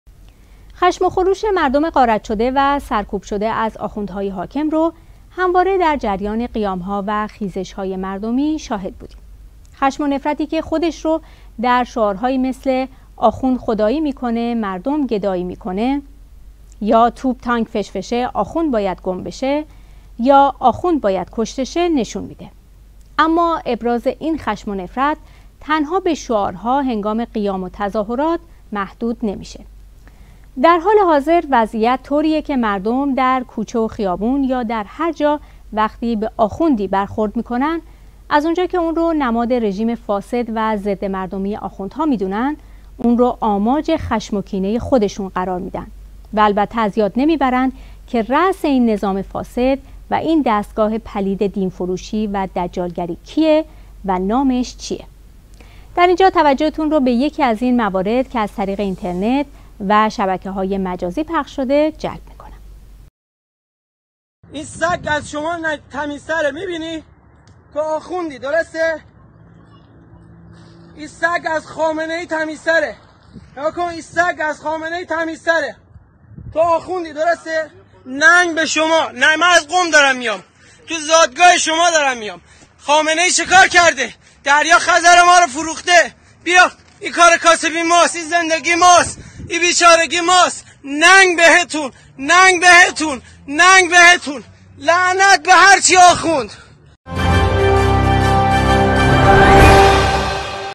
ننگ و لعنت به هرچی آخوند -برگرفته از اینترنت
حمله و فحش به یک آخوند
خشم و خروش مردم غارت شده و سرکوب شده از آخوندهای حاکم را همواره در جریان قیامها و خیزشهای مردمی شاهد بوده‌ایم. خشم و نفرتی که خودش را در شعارهایی مانند آخوند خدایی می‌کند، مردم گدایی می‌کنند یا توپ تانک فشفشه آخوند باید گم بشه یا آخوند باید کشته شه نشان می‌دهد. اما ابراز این خشم و نفرت تنها به شعارها هنگام قیام و تظاهرات محدود نمی‌شود؛ در حال حاضر وضعیت طوری است که مردم در کوچه و خیابان یا در هر جا وقتی به آخوندی برخورد می‌کنند، از آنجا که او را نماد رژیم فاسد و ضدمردمی آخوندها می‌دانند، او را آماج خشم و کینة خود قرار می‌دهند و البته از یاد نمی‌برند که رأس این نظام فاسد و این دستگاه پلید دین‌فروشی و دجالگری کیست و نامش چیست؟. در اینجا توجهتون رو به یکی از این موارد که از طریق اینترنت و شبکه‌های مجازی پخش شده، جلب می‌کنم:
«این سگ از شما تمیزتره می‌بینی؟ تو آخوندی درسته؟ این سگ از خامنه‌ای تمیزتره، نگاه کن این سگ از خامنه‌ای تمیزتره. نه درسته؟... ننگ به شما، نه من از قم دارم می‌آم، از زادگاه شما دارم می‌آم.
خامنه‌ای چی کار کرد؟ دریای خزر ما رو فروخته، بیا این کار و کاسبی ماست، این زندگی ماست؛ این بیچارگی ماست، ننگ بهتون لعنت به هر چی آخوند».